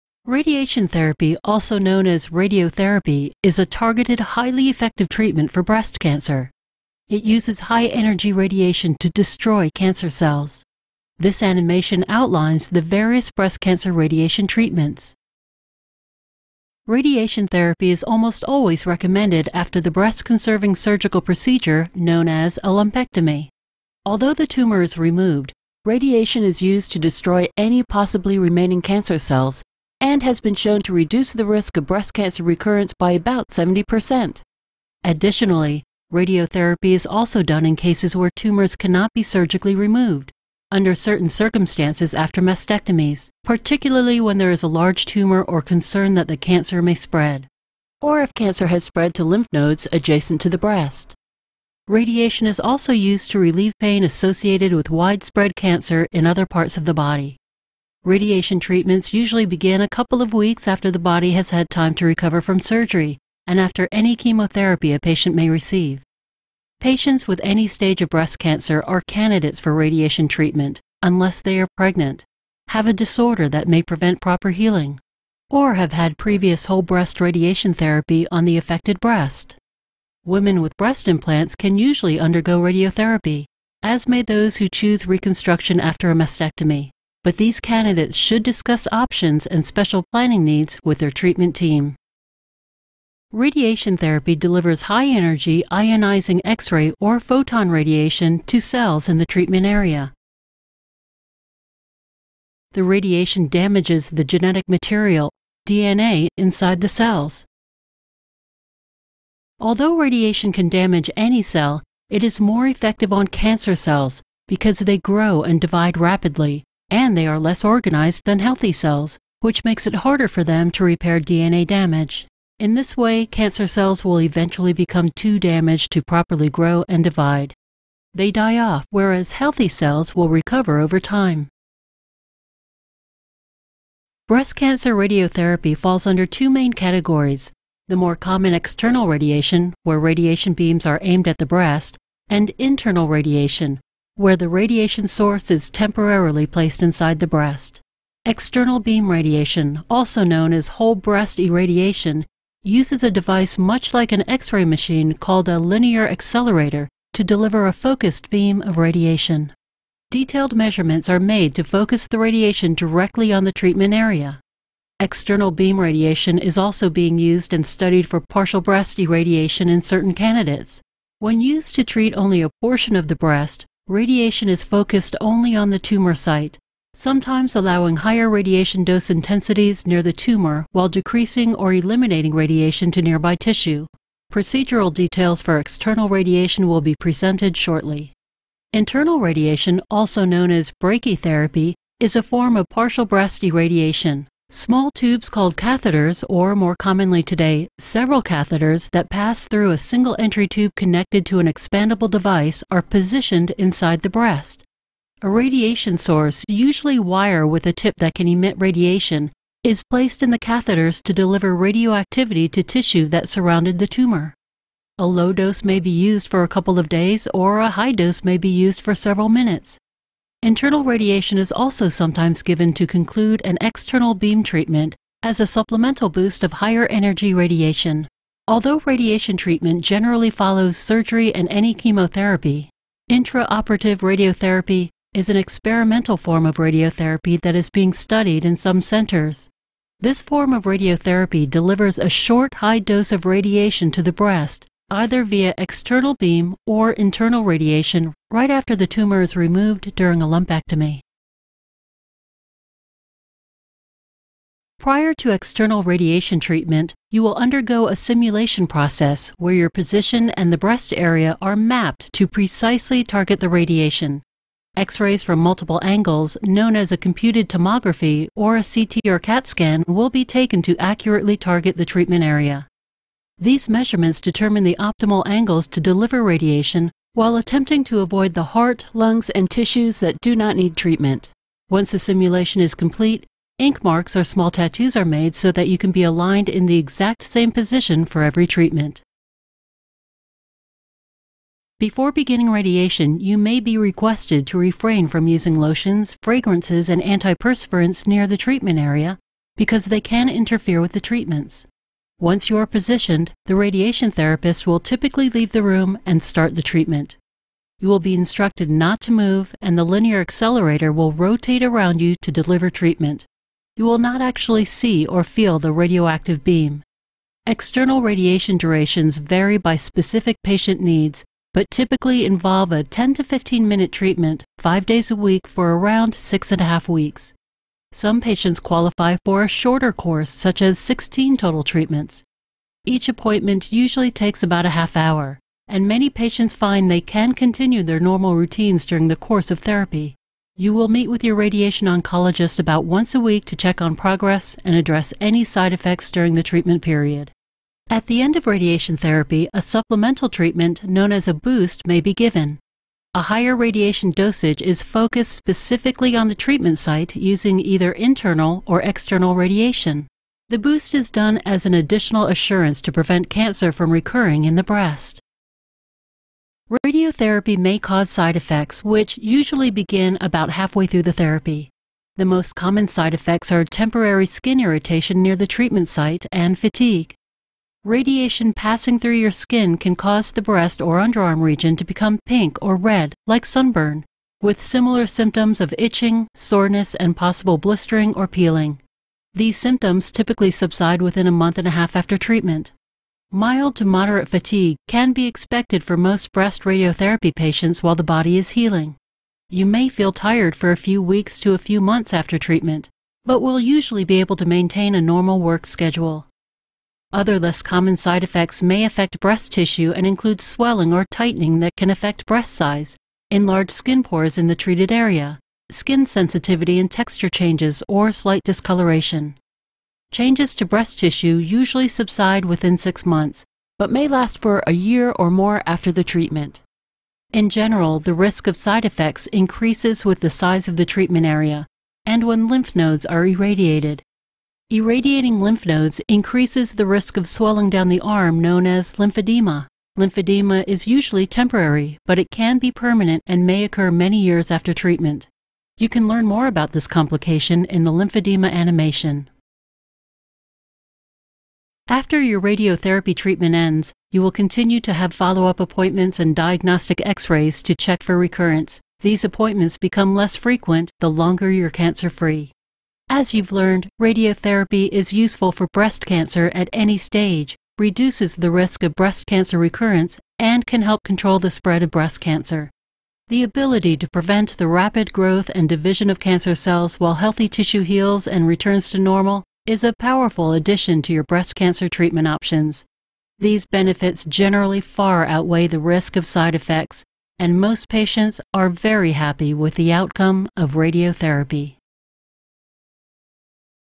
How radiation is used to destroy Breast Cancer cells [7 of 8]
Radiation therapy (radiotherapy) is a targeted and highly effective treatment for breast cancer. Learn about the various radiation methods used for treating breast cancer.